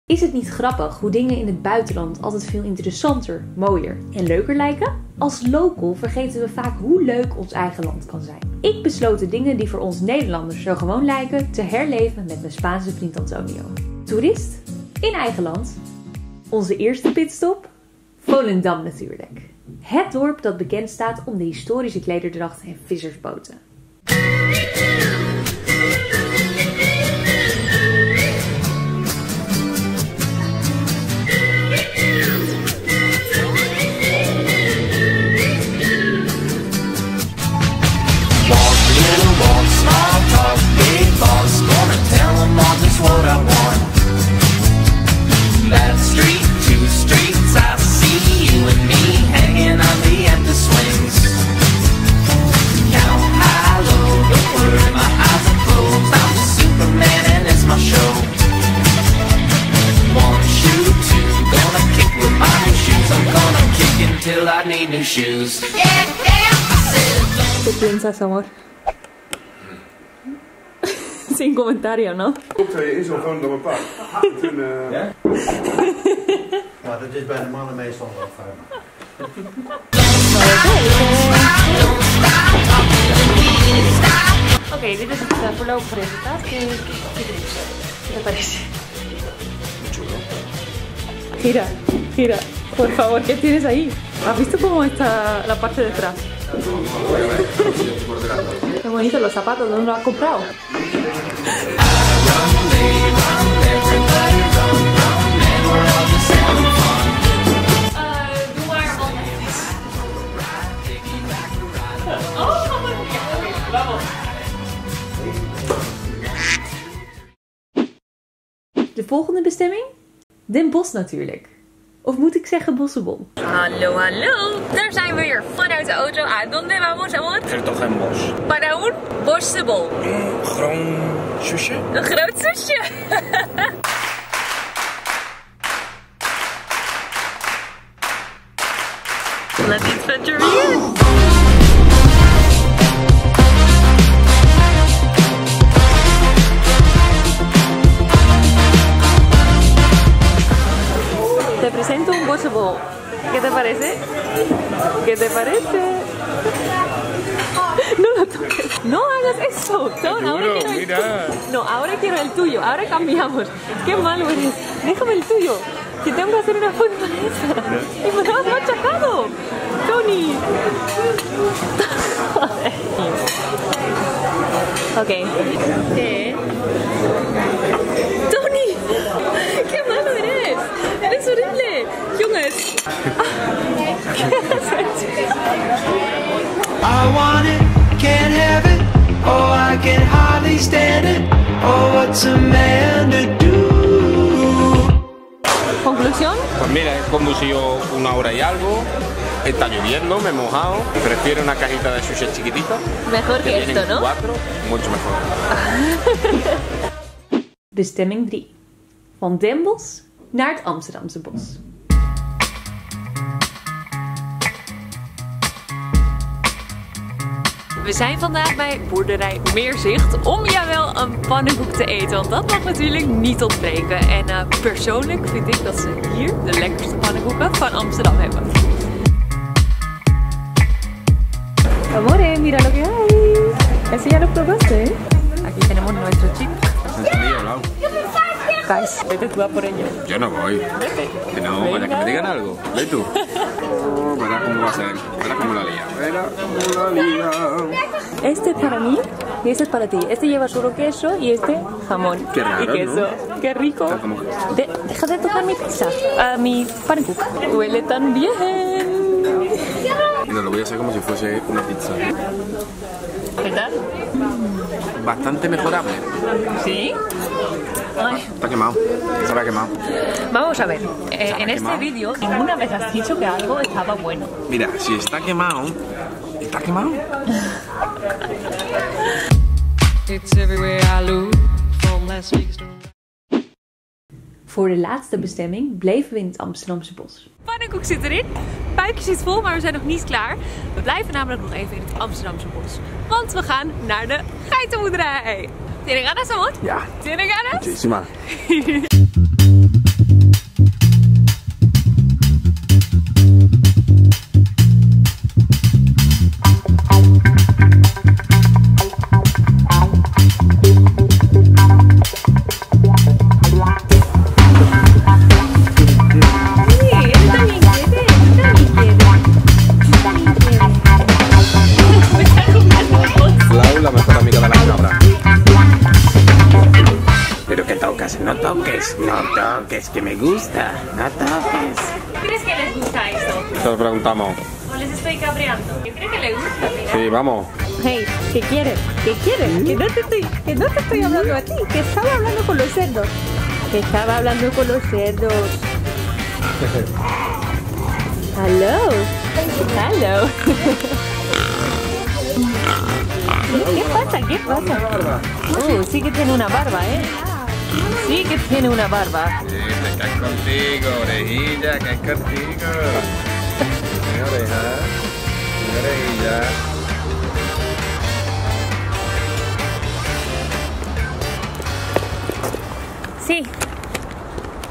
NEDERLANDSE DINGEN DOEN MET EEN SPANJAARD
3 dagen, 4 plekken, 1 Spanjaard.

Laura Brijde
Passeerdersgracht 18
1016XH Amsterdam